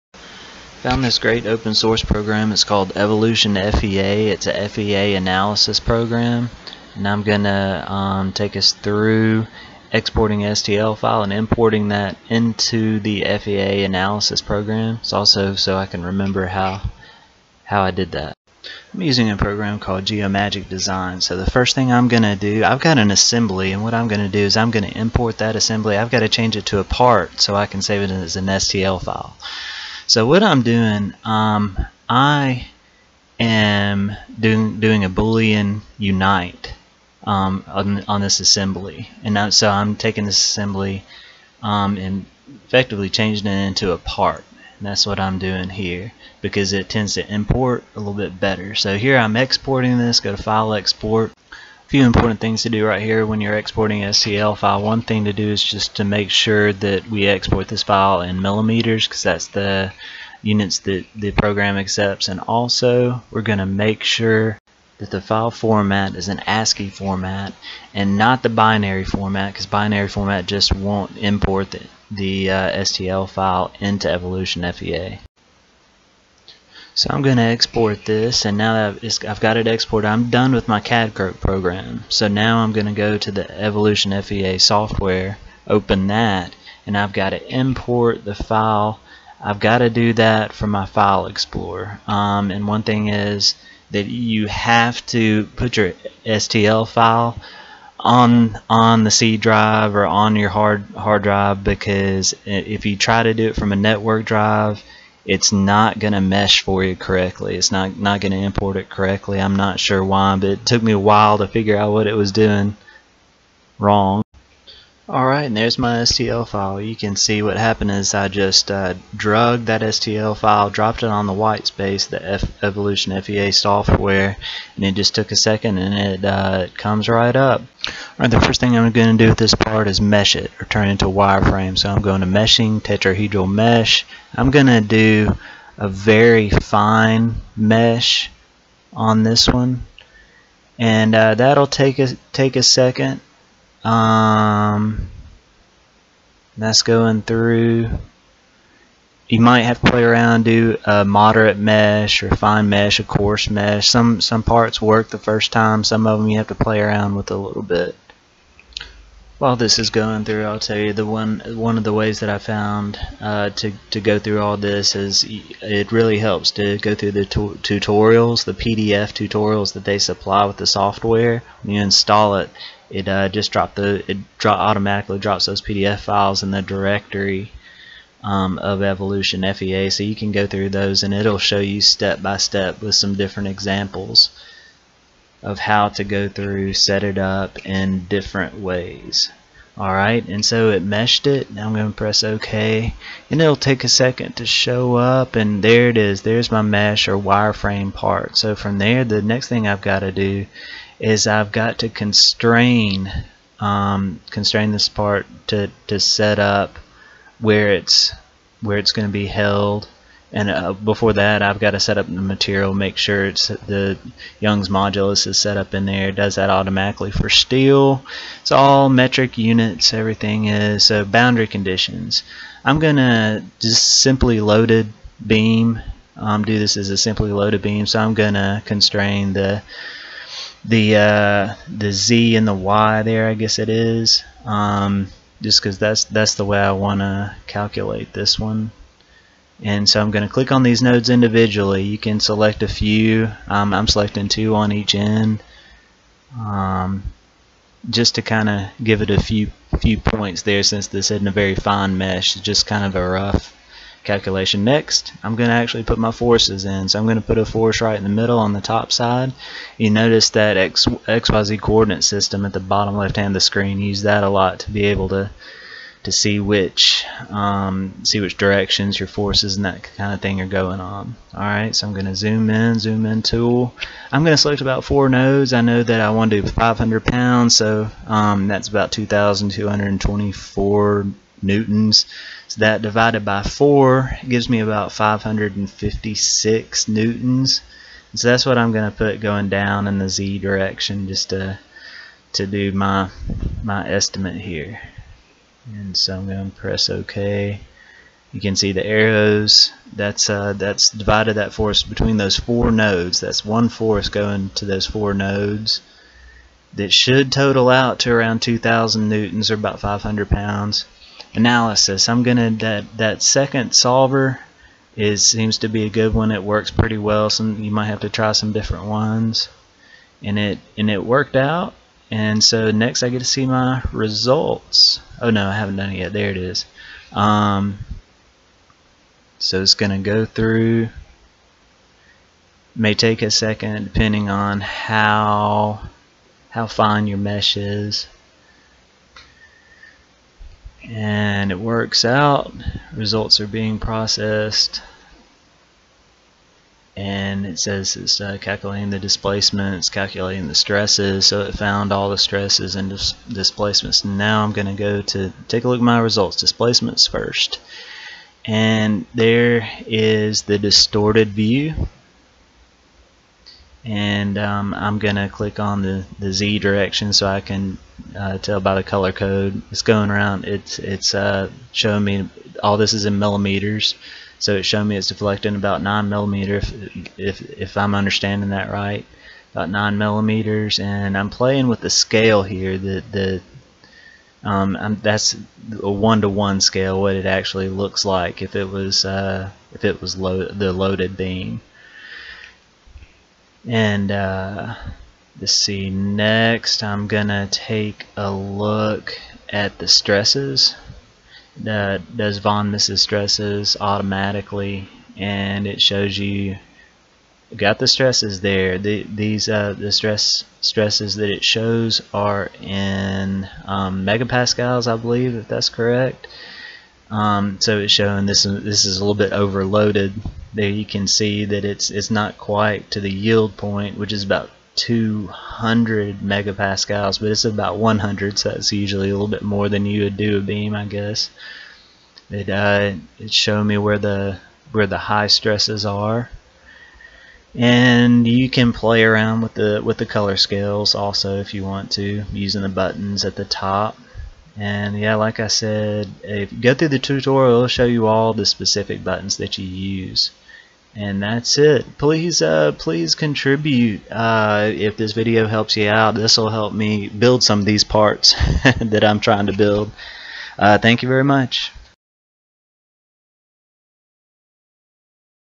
Geomagic Design to Evolution FEA (free open source stress and bending analysis software)
This video shows the toolpath to go from a 3d model cad design to Evolution FEA software.  This is in order to estimate stresses and bending on parts due to forces.  It can also do temperatures, I believe.

FEA - Finite Element Analysis

Bitcoin address:
13cx8m4fHcCF94N32Lw8xbpLjtHDGHrfZU